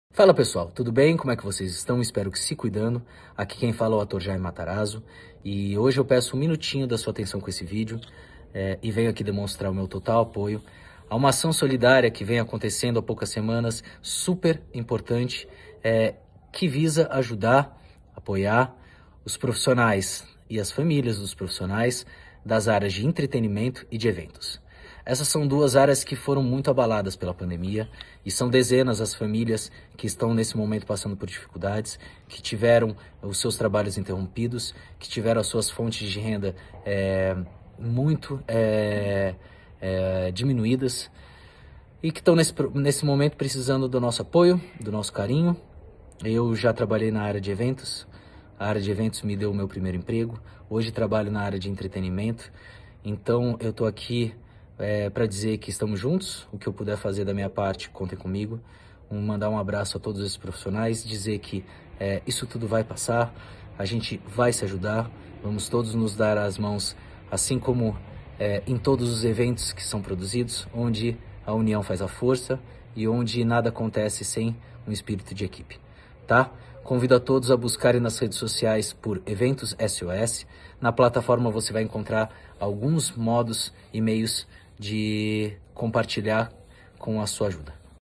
Jayme Matarazzo - Ator
Muito obrigado Jayme Matarazzo pela generosidade e carinho com a Ação Solidária

Doe, compartilhe, colabore!